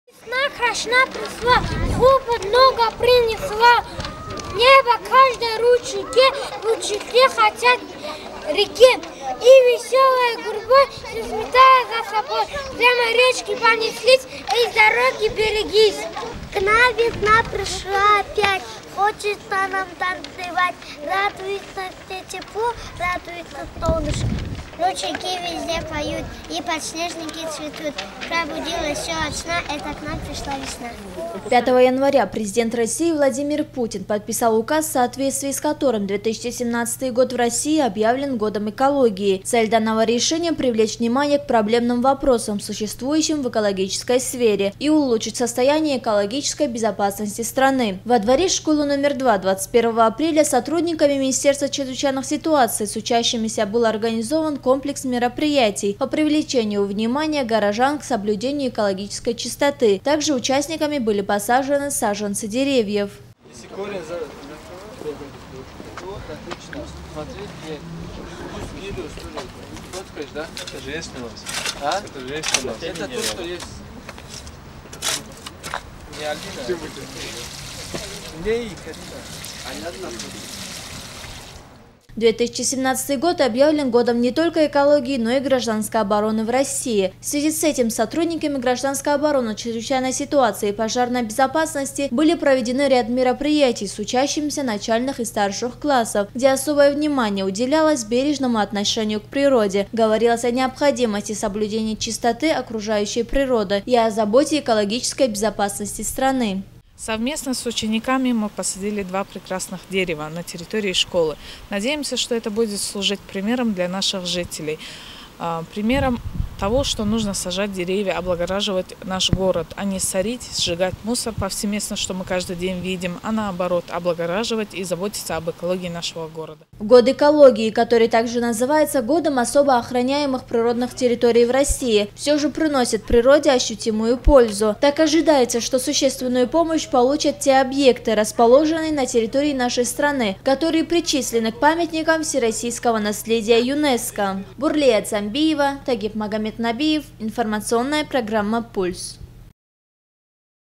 Год экологии отметили в СОШ№2
В России 2017 год объявлен годом Экологии и Гражданской обороны. В связи с этим во дворе школы №2 21 апреля сотрудниками МЧС и учащимися был организован комплекс мероприятий по привлечению внимания горожан  к соблюдению экологической чистоты, также участниками были посажены саженцы деревьев.